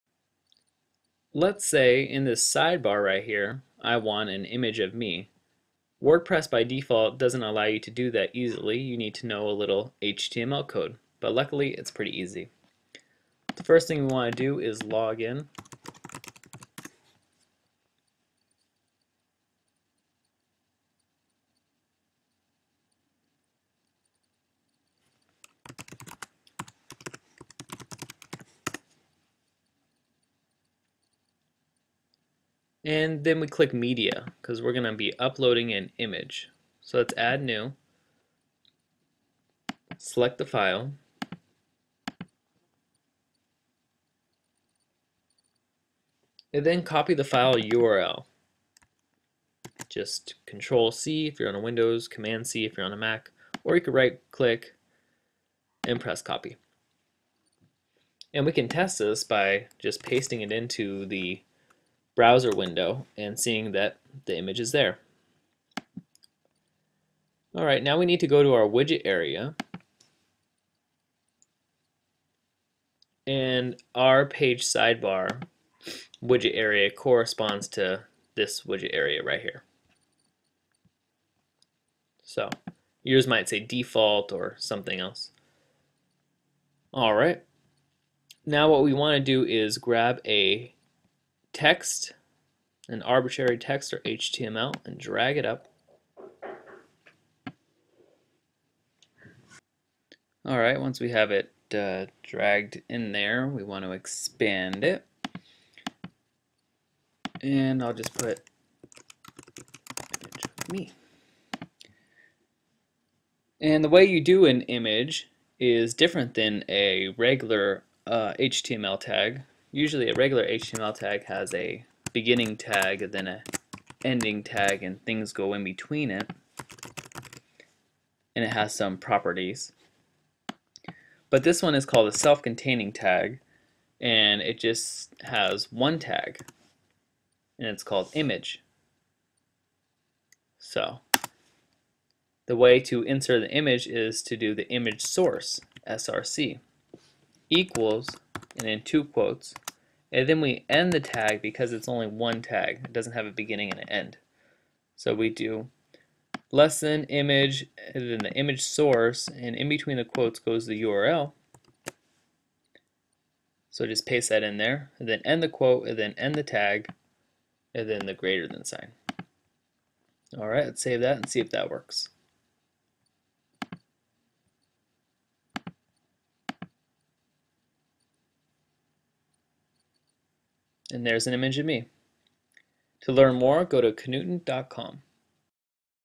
Insert Image Into The Sidebar
Tutorial on how to insert an image into the wordpress sidebar widget area.